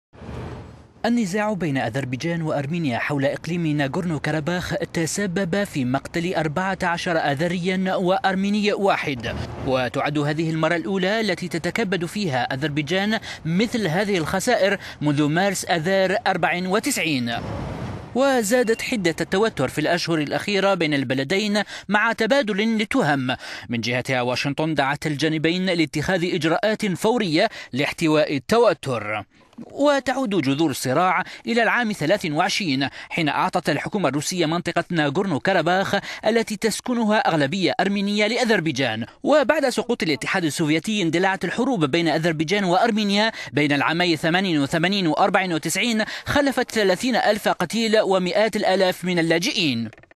الصراع بين أذربيجان و أرمينيا حول إقليم ناغورنو كرباخ خلف 15 قتيلا
النزاع بين أذربيجان و أرمينيا حول إقليم ناغورنو كرباخ تسبب في مقتل 14 أذريا و أرميني واحد
و تعد هذه المرة الأولى التي تتكبد فيها أذربيجان مثل هذه الخسائر منذ مارس/ آذار 1994
و زادت حدة التوتر في الأشهر الأخيرة بين البلدين مع تبادل للتهم
من جهتها واشنطن دعت الجانبين لاتخاذ إجراءات فورية لاحتواء التوتر

و تعود جذور الصراع إلى عام 1923 حين أعطت الحكومة الروسية منطقة ناغورنو كرباخ  التي تسكنها أغلبية أرمينية لأذربيجان  ، و بعد سقوط الاتحاد السوفياتي اندلعت الحروب بين اذربيجان و أرمينيا بين 1988 و 1…